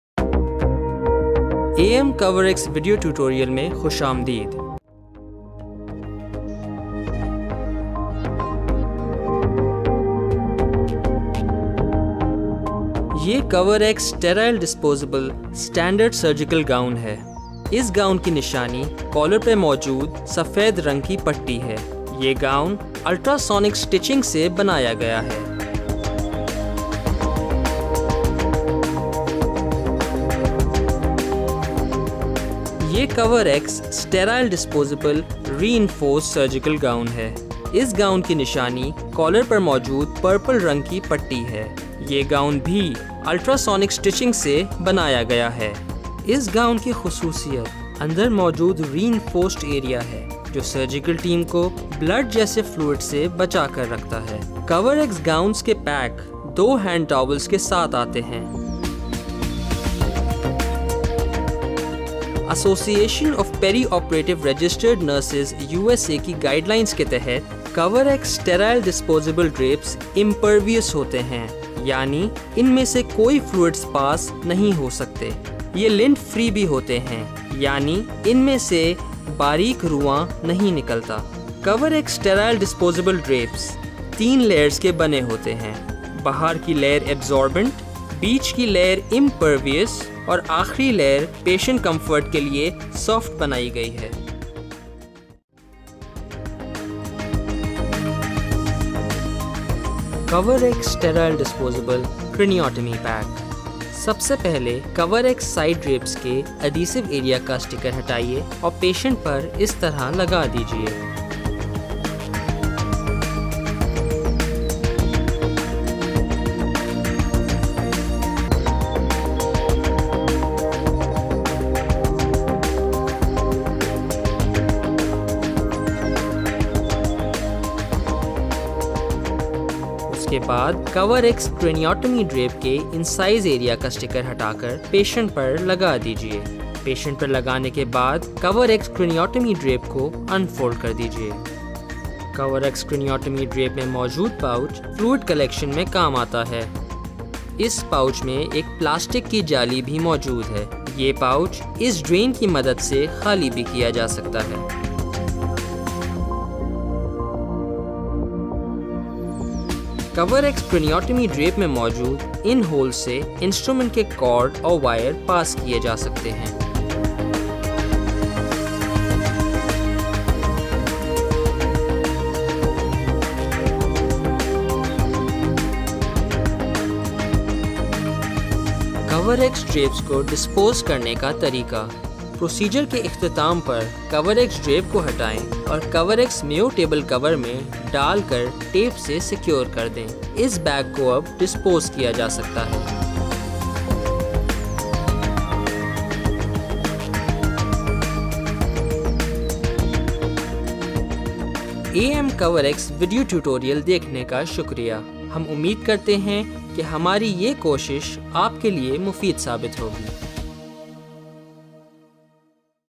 Cover X® Sterile, Disposable Drapes (Neurosurgery) and Gowns Video Tutorial
Cover X® Sterile, Disposable Drapes (Neurosurgery) and Gowns Video Tutorial covers draping procedures for craniotomy cases in the OR. This tutorial has been produced keeping internationally accepted standards in mind.

Location: Dow University Hospital, Karachi

Note: We would like to thank the administration of Dow University Hospital for allowing us to use its OR.

Produced by: A.M. Distributors